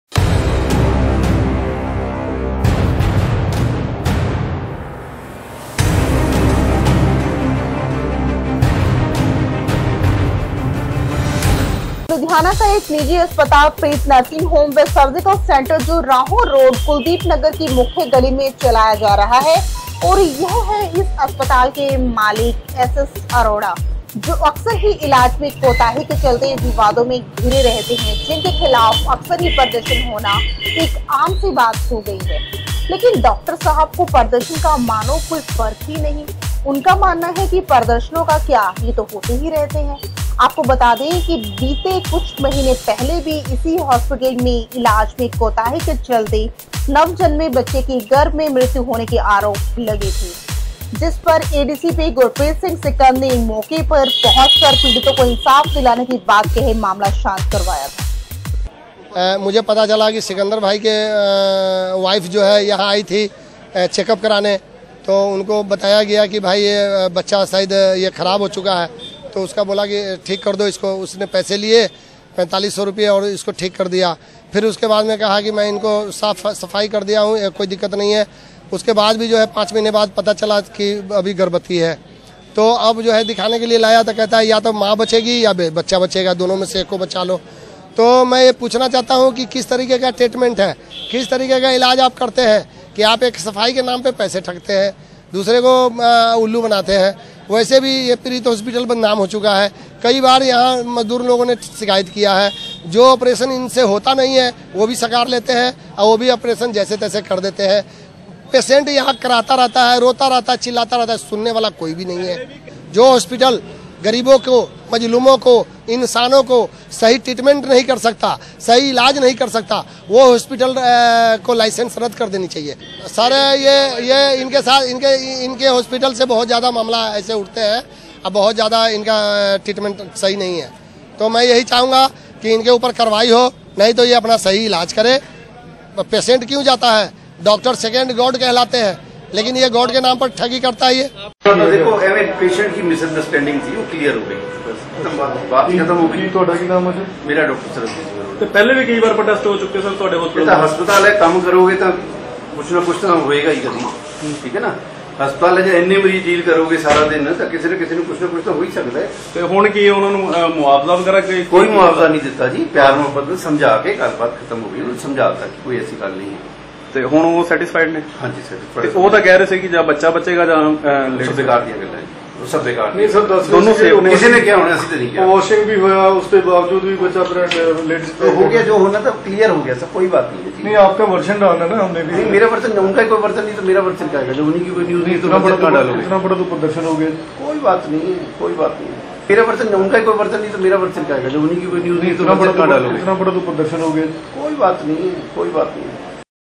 The wire tv subscribe channel हॉस्पिटल लापरवाही का मामला
The wire tv is the news channel in world.
The wire tv is the best channel प्रीत हॉस्पिटल एक वार फ़िर सुर्खियों में, फ़िर लगे इलाज़ में कोताही के आरोप, चार महीने के भीतर दूसरा लापरवाही का मामला